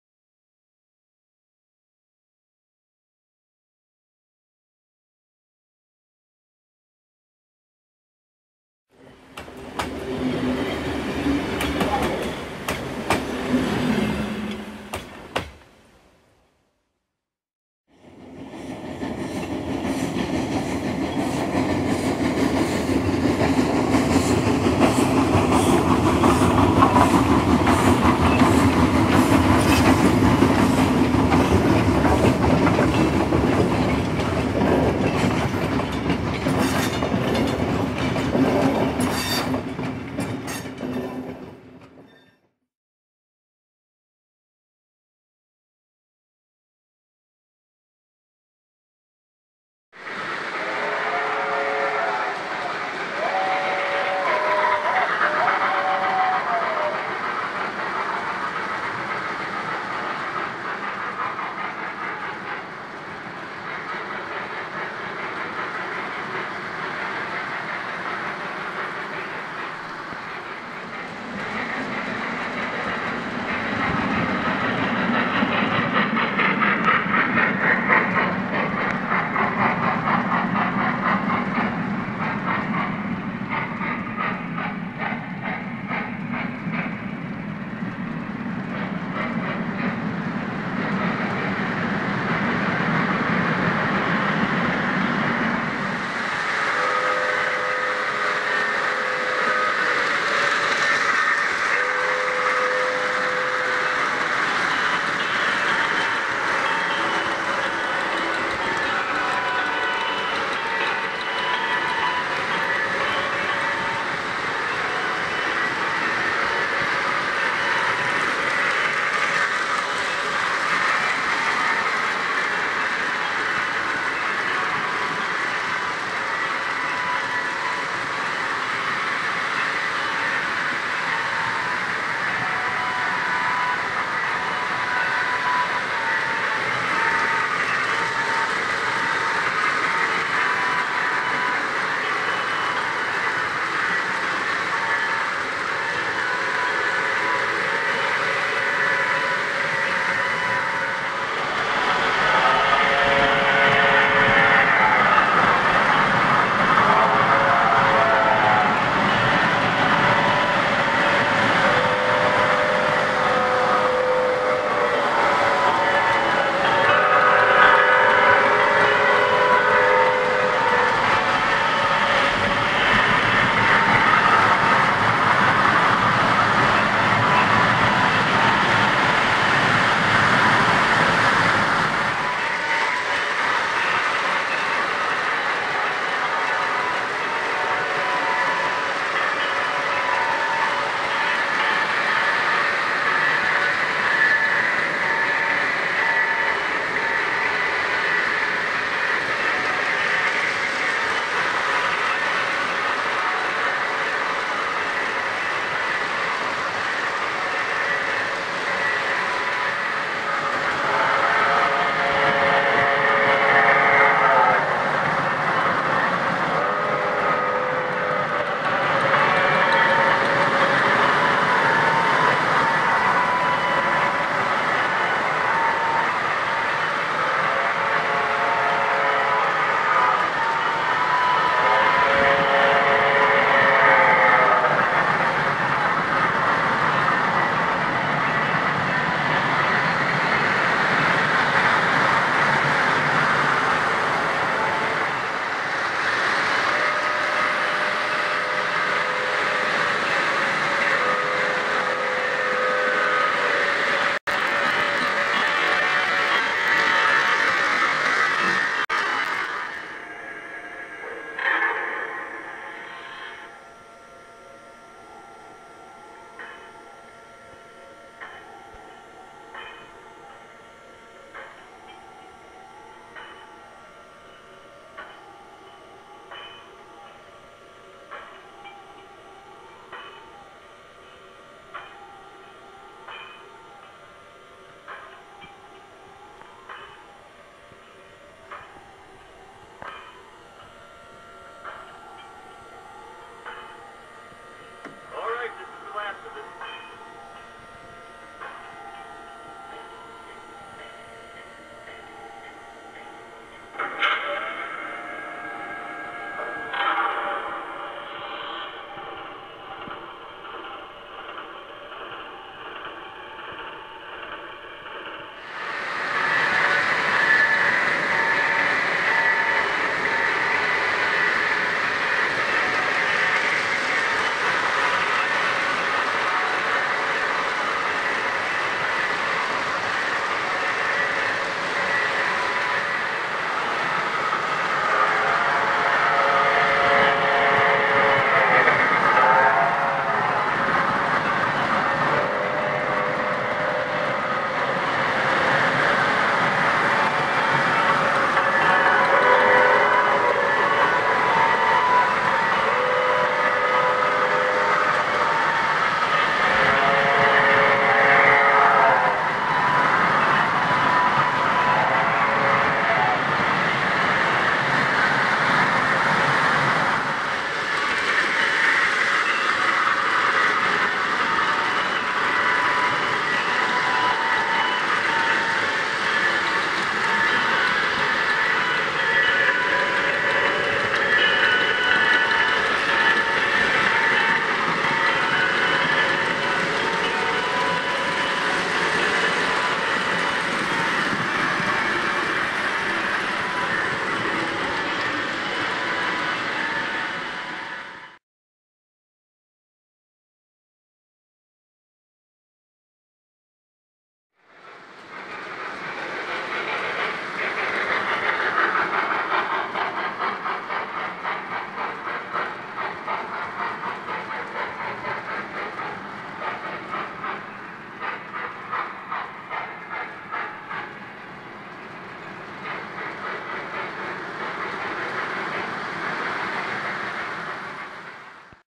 MTH Limited Edition Coors Silver Bullet O-GaugeOn-Line HD
My brother flew in from Colorado to visit for a week.  Seemed like a good time to put our Denver & Rio Grande Western Berkshire on the tracks, set up a colorful freight - complete with the great looking D&RG caboose, AND pull out the sharp looking Coors Silver Bullet train for us both to enjoy!  MTH made a real winner, which sounds great, especially when stopped and the cars open to reveal its cargo of 'cold ones'.   He was blown away by both the 'Protosounds' and the sets 'cool' operation in every way.  O-gauge model trains set up and operated by the Silver Rail Club in Virginia by Peter and Hans Martens.